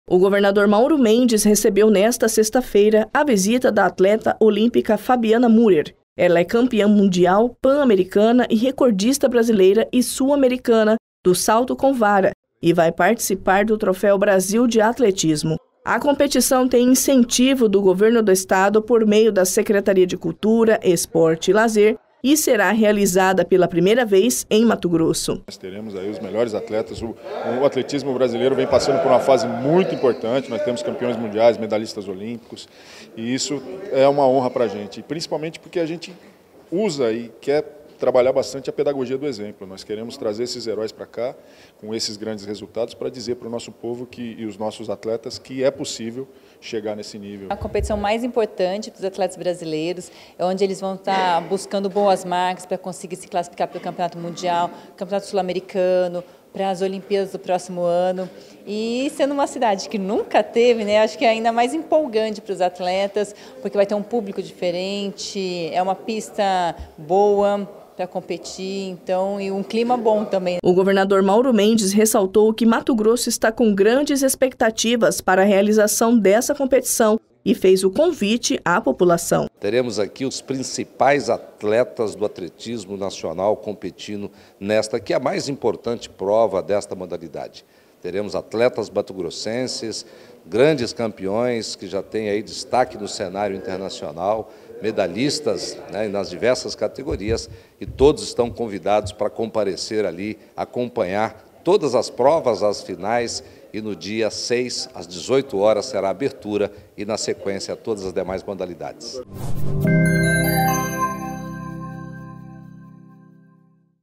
Governador recebe campeã olímpica de atletismo que participará do Troféu Brasil em MT
Com o fomento da Secretaria de Estado de Cultura, Esporte e Lazer, o Troféu Brasil de Atletismo é o maior evento da categoria e será realizado pela 1º vez em Mato Grosso nos dias 6 a 9 de Julho no Centro Olímpico de Treinamento da Universidade Federal de Mato Grosso.